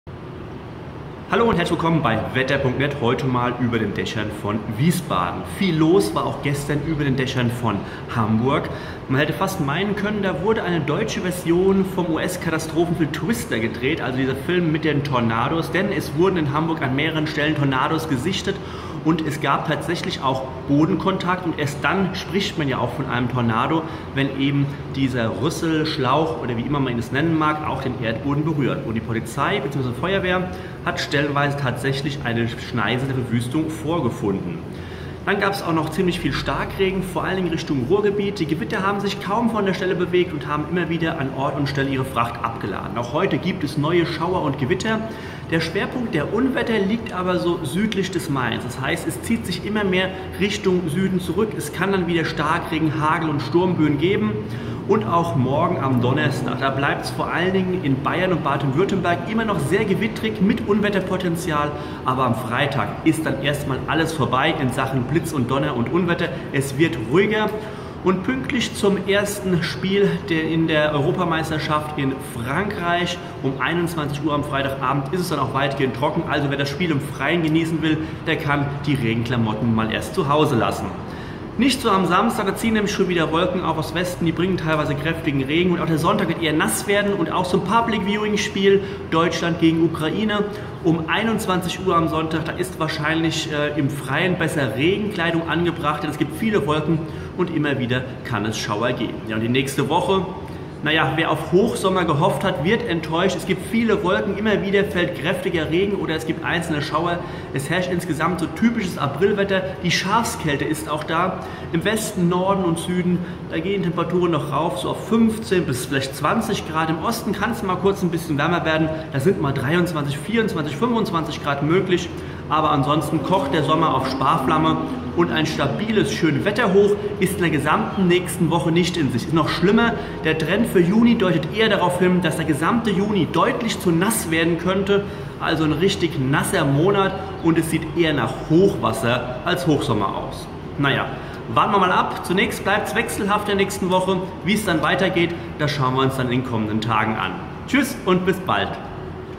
Tornado über Hamburg - wie geht´s weiter? (Mod.: Dominik Jung)
Das aktuelle Deutschlandwetter vom 8. Juni 2016.